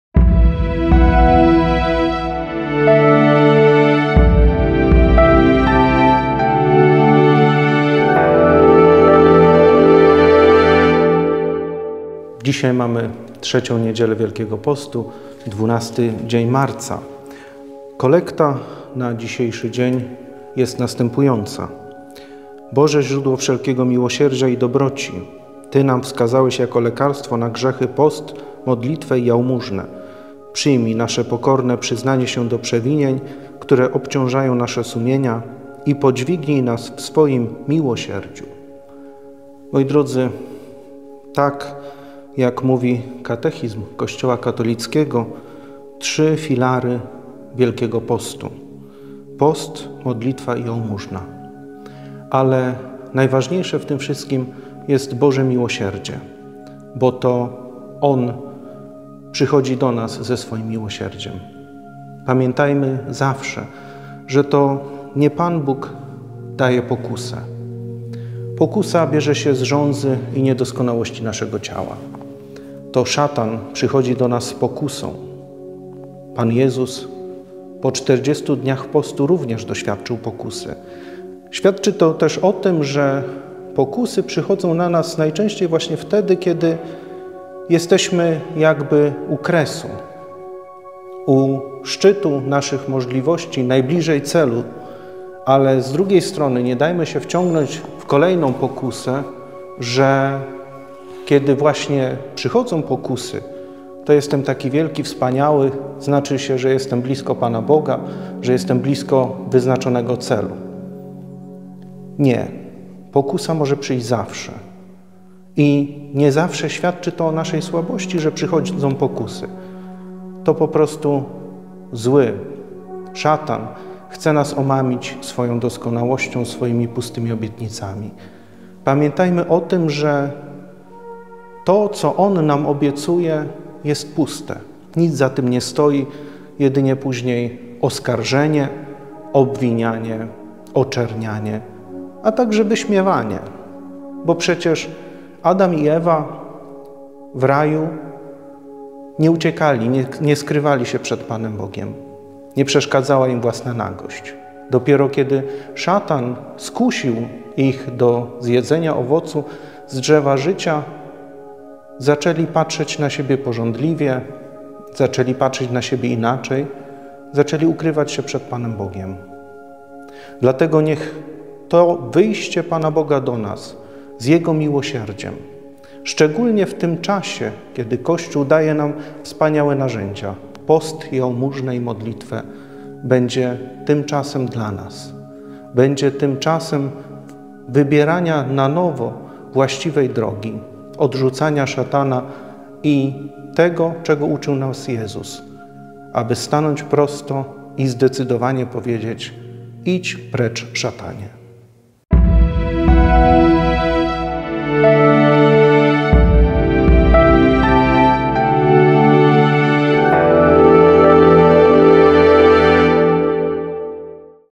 2023-03-12 - Kolekta na Wielki Post - o Michał Bortnik
❗❗❗ KOLEKTA NA DZIŚ: Boże, źródło wszelkiego miłosierdzia i dobroci, Ty nam wskazałeś jako lekarstwo na grzechy post, modlitwę i jałmużnę, przyjmij nasze pokorne przyznanie się do przewinień, które obciążają nasze sumienia, i podźwignij nas w swoim miłosierdziu.

Codzienny komentarz do kolekty na każdy dzień Wielkiego Postu 2023